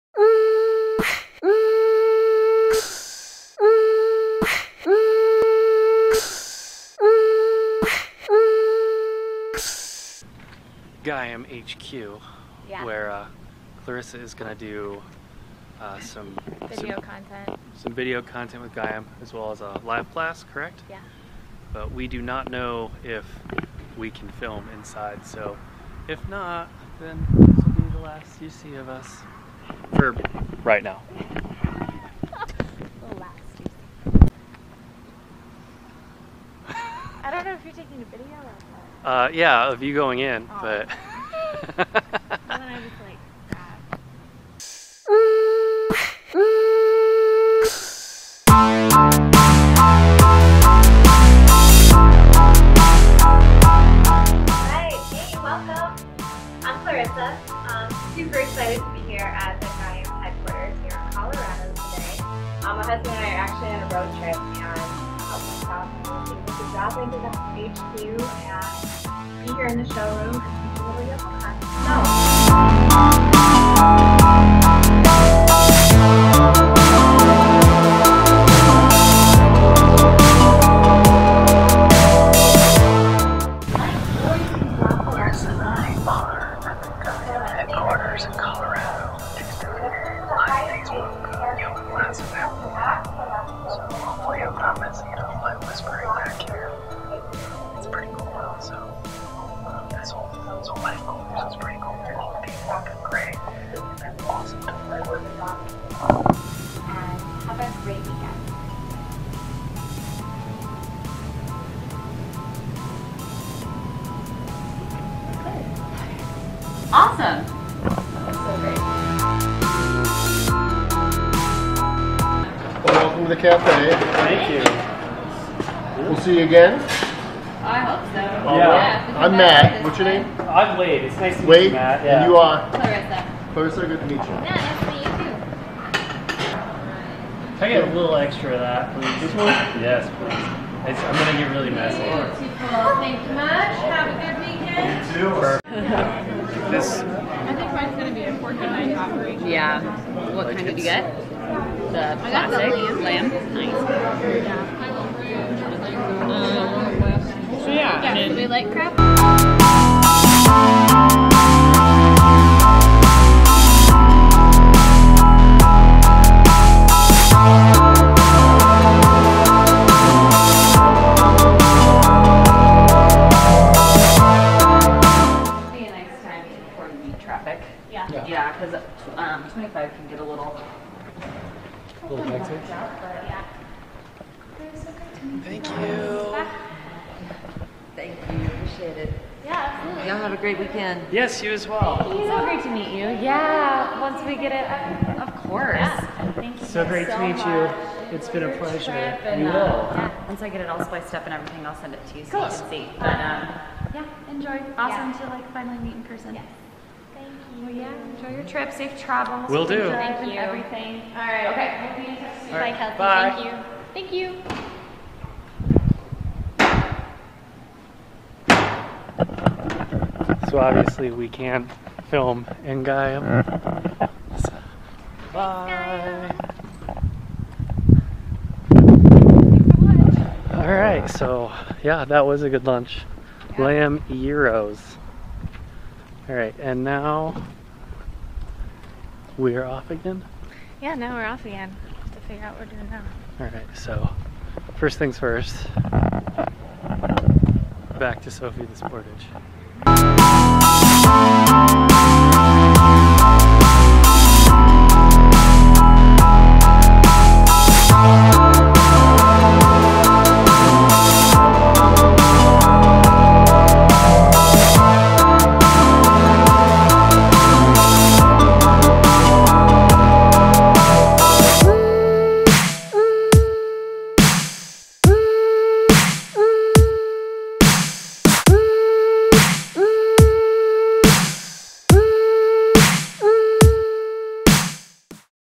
Road Trip Day 4 (The One with All the Yoga)
Day 4 finds us at Gaiam headquarters in Colorado, where Clarissa teaches a Facebook live class, we do some filming, and we eat lamb gyros in the cafe. Pretty cool. Thanks Gaiam.